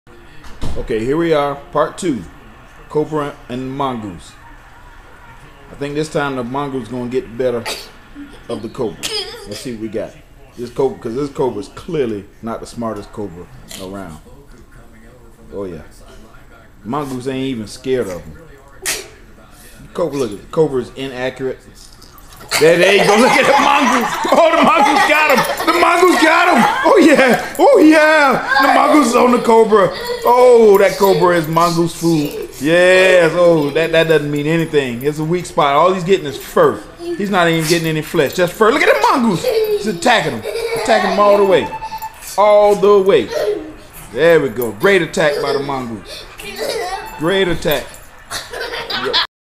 Cobra vs. Mongoose fight to the death 2
This time the mongoose gets his revenge.